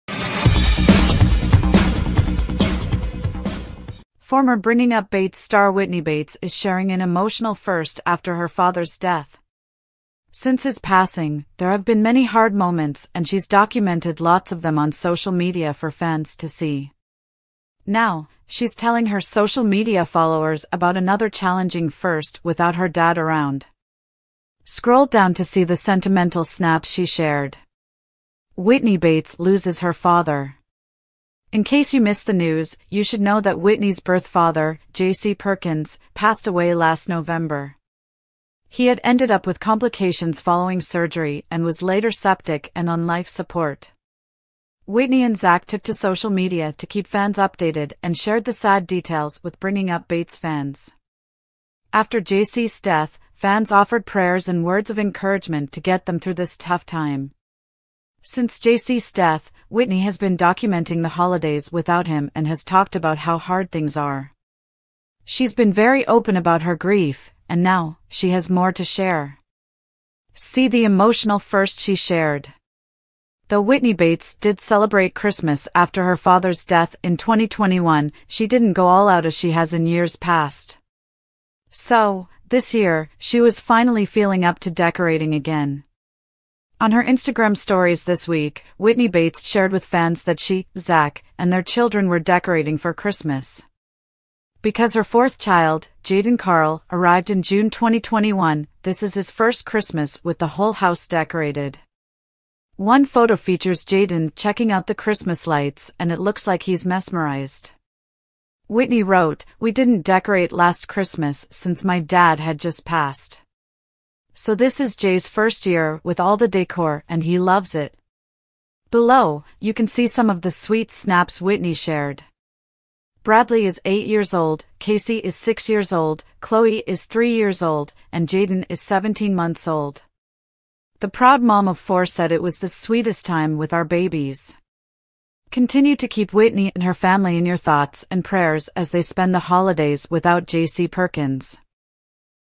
Whitney Bates Shares Emotional First After Father’s RIP
Whitney Bates, who was a star on Bringing Up Bates, is opening up about her grief for the first time.She has documented a lot of the difficult times since his death on social media for fans to see.Presently, she's informing her virtual entertainment adherents concerning one more testing first without her father around.To view her sentimental photos, scroll down.